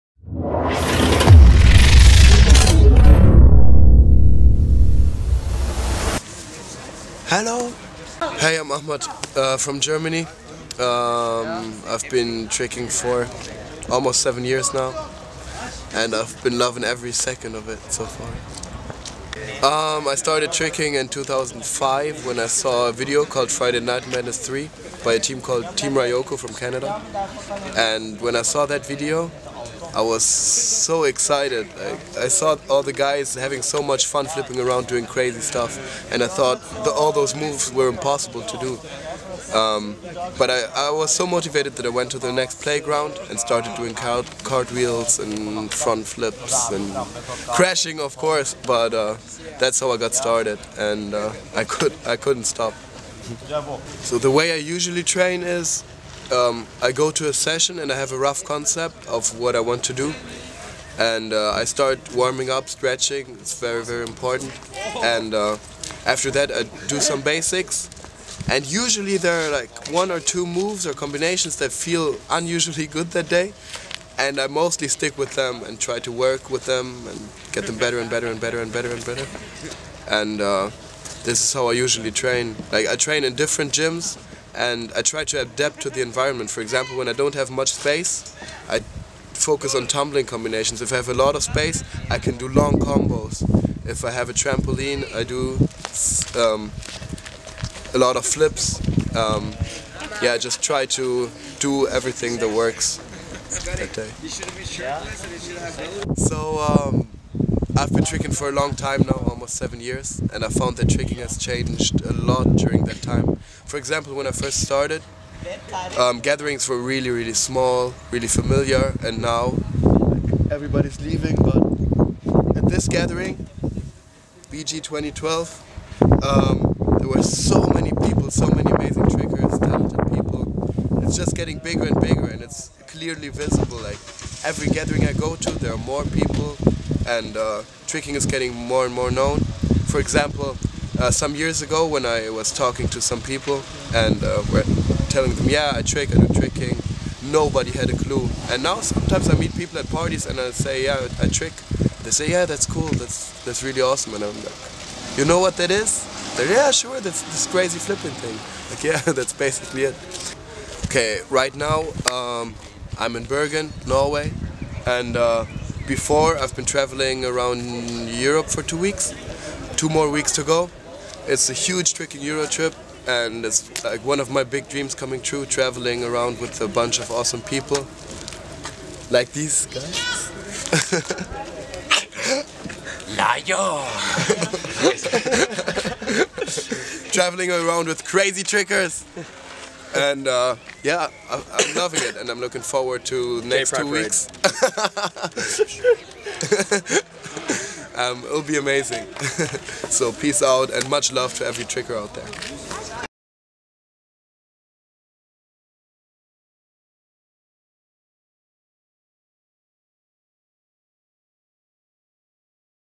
Ahmad Frenzel Interview - Invincible Tricking
A heart to heart with Germany's Ahmad Frenzel at Bergen Gathering 2012